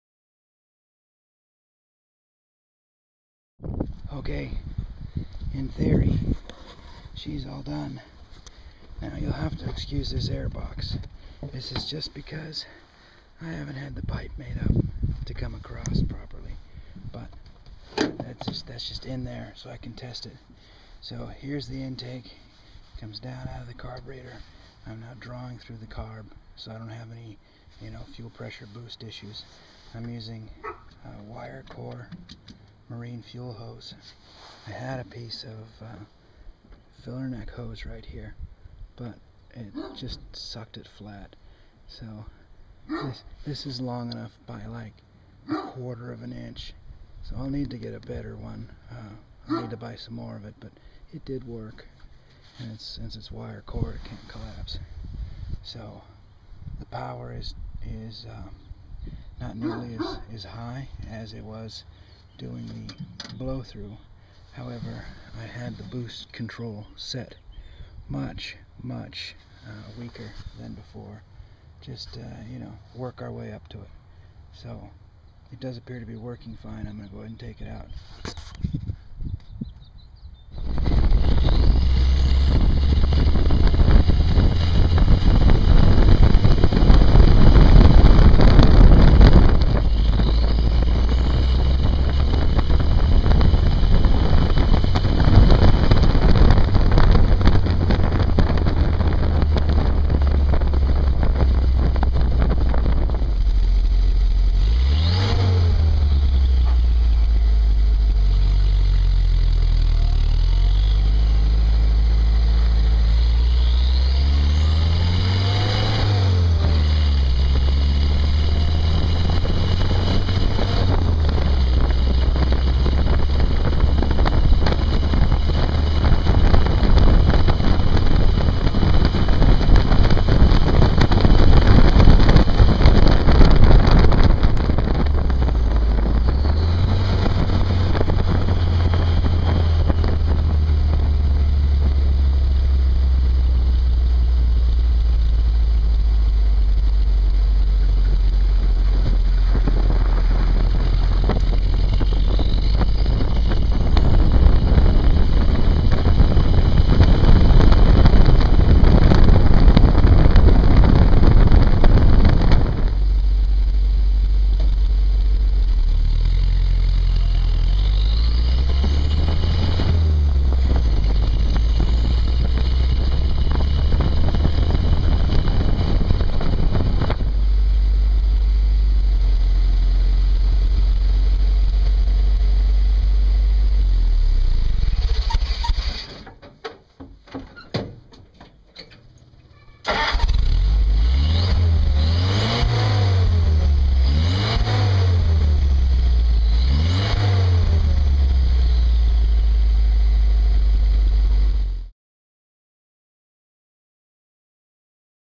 Jeep Turbo Finished
I will do a better video once I get the extra airbox out of the way and when I heal up some.  Turbo is in and working in it's final configuration.

If you have questions I kept a build log and you are welcome to read, question and comment there.